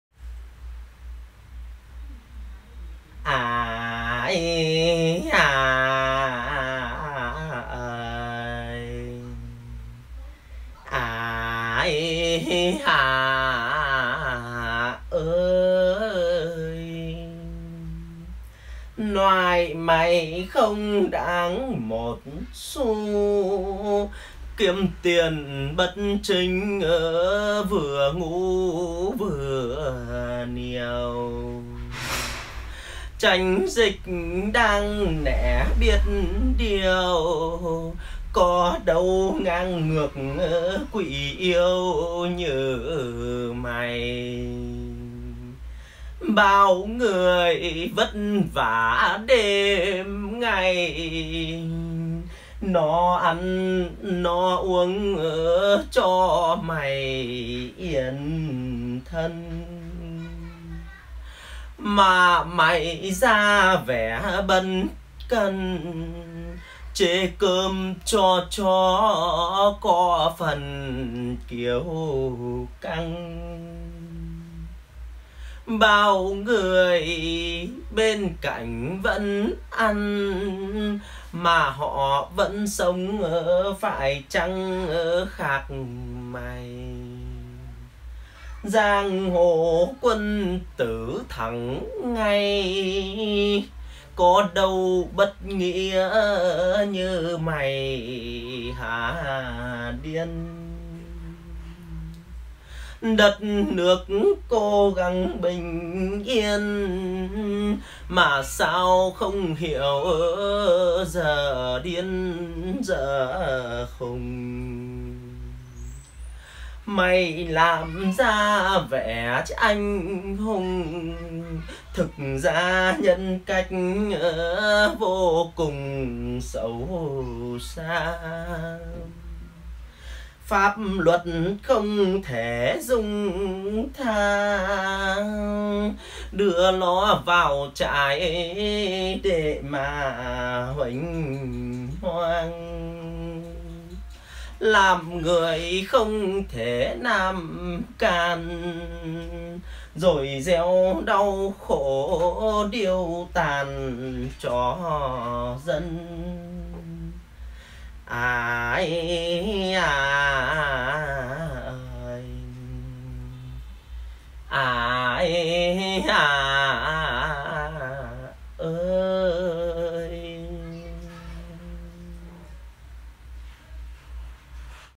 Tế ch.ửi giang hồ hổ báo chê “cơm ở khu cách ly chỉ để cho chó ăn...” của Mạnh Hoàng Vlog
TỔNG HỢP VIDEO NỔI BẬT YOUTUBE CỦA MẠNH
Cảm ơn mọi người nhiều lắm!
manhhoangvlog, thanhte, Mạnh Hoàng Vlog, Manh Hoang Vlog, thánh tế